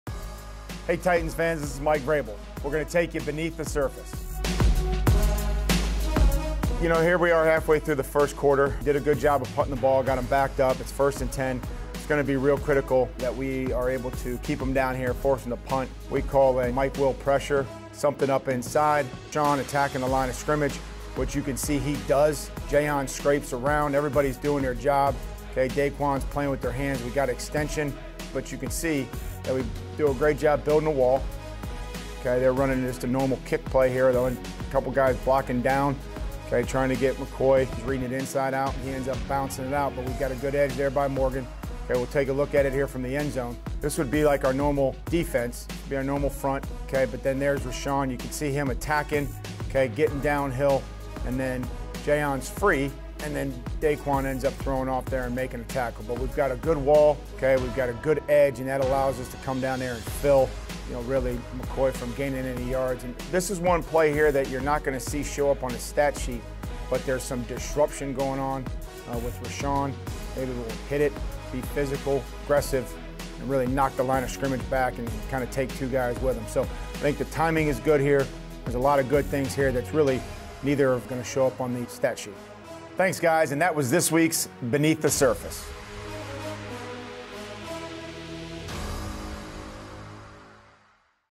Beneath the Surface with Mike Vrabel
Titans head coach Mike Vrabel breaks down a big defensive stop against the Bills in this week's edition of "Beneath the Surface."